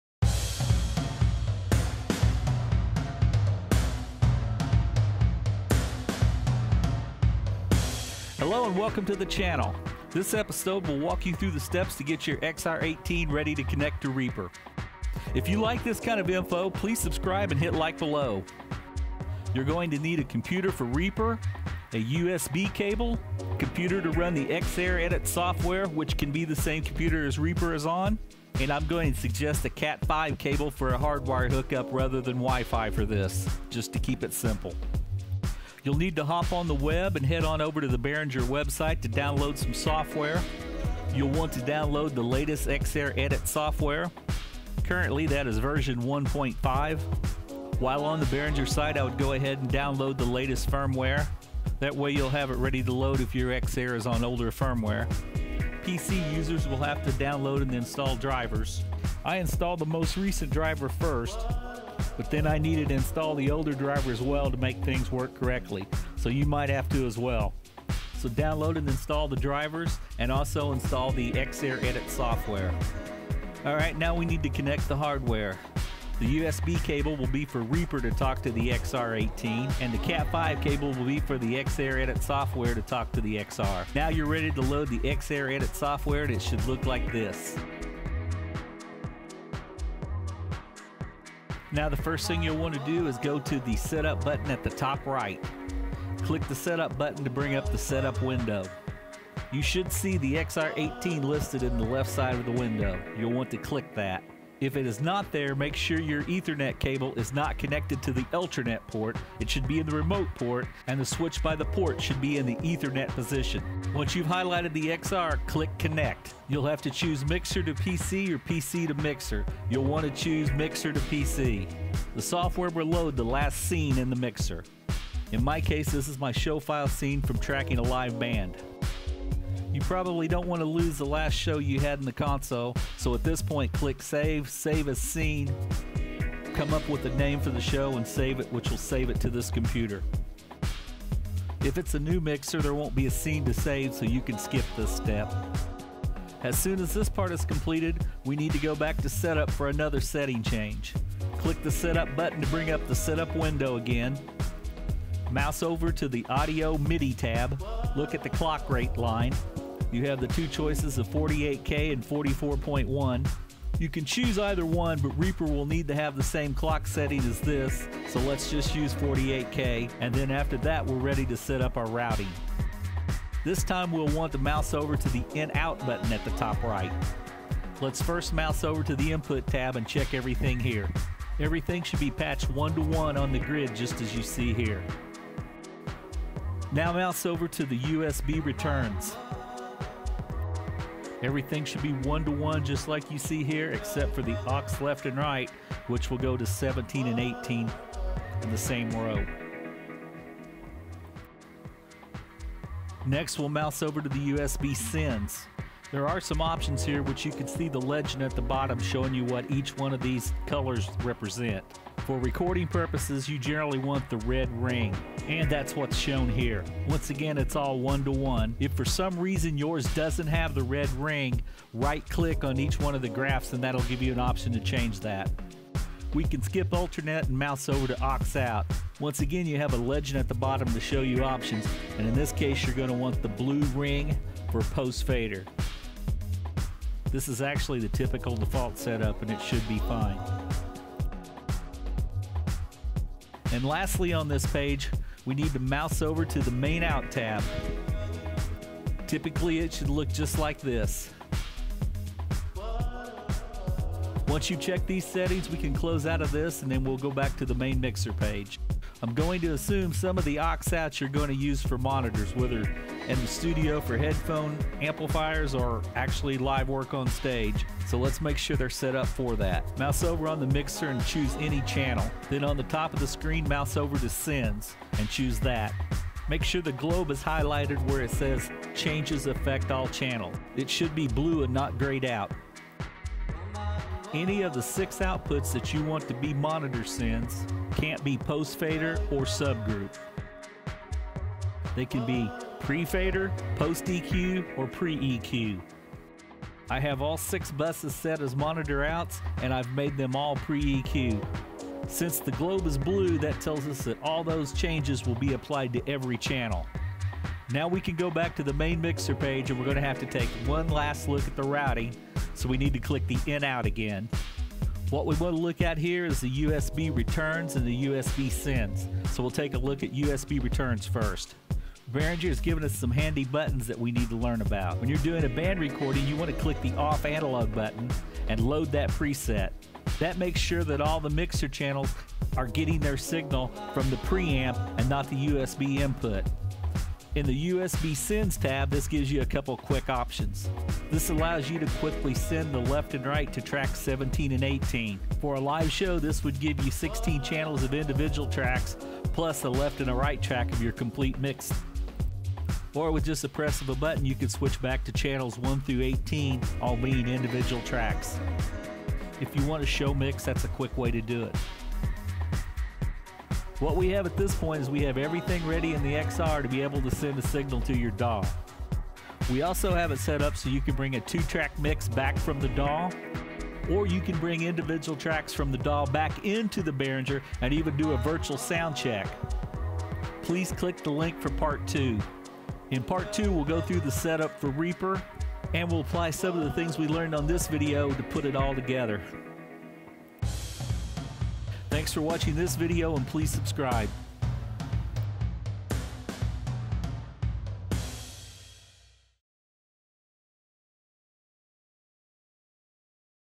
Connecting A Behringer XR18 To Reaper: Part One Setting Up The Behringer XR18
Use your XR18 or Midas MR18 as a USB audio interface. X-Air recording tutorial.

There's now a full version of these videos, updated editing, and no backing music track. Click here for this newer version:

10' (3 meter) USB Cable on Amazon

Cat 5E (25') Cable on Amazon
How to connect your XR18 to Reaper.

This Five Minute Tech Tip wouldn't all fit in 5 minutes. In fact, both part 1 and 2 ran a little long. But there's a lot of information to go through.

I used a PC for this project, but most of this is basically applicable to the Mac versions of the software as well.

Part one focuses on the hardware prep, and configuring the XR18 via X-Air Edit software.
One computer, assuming it has enough horsepower, can run both Reaper and X-Air Edit. Or two separate computers can be used instead with one computer running X-Air Edit, and one running Reaper.
You can still mix via iPad or Tablet and don't need the X-Air Edit to run concurrently once you have things configured. If you're comfortable enough with the presets and routing options in the iPad/tablet then you can do much of this from there. But the X-Air Edit software is easier to learn the techniques and flow, and to simply learn and understand the concepts and things you're looking for.

The Reaper computer will need a USB connection. The X-Air Edit computer will need a Cat5e connection (or wifi). For the first time setting all of this up I recommend the hardwired Cat5e connection to avoid any pitfalls of wifi setup. It's one less wildcard variable while getting this to work for the first time. Plus, the X-Air Edit software is how you update the firmware on the XR18, and for anyone updating firmware at some point in the future, I feel more comfortable if they use a hardwired connection.

Both connections (USB and Cat5e) are needed, even if the same computer is handling both software programs.

For PC users running 2 PC's for this, the Reaper PC is the computer that needs the Behringer USB drivers installed on it.

The second video- Part Two: Setting Up Reaper, focuses primarily on configuring Reaper to communicate with the XR18 and allowing the XR18 to function as an 18 channel USB interface with 16 mic pres, and 2 aux line inputs.  The 2nd half of the video is the next step beyond that and focuses on setting up Reaper for the ability to do things like virtual soundchecks, and even the ability for mixing tracks within the XR18 itself rather than 'in the box' on the Reaper machine, should anyone want to do that.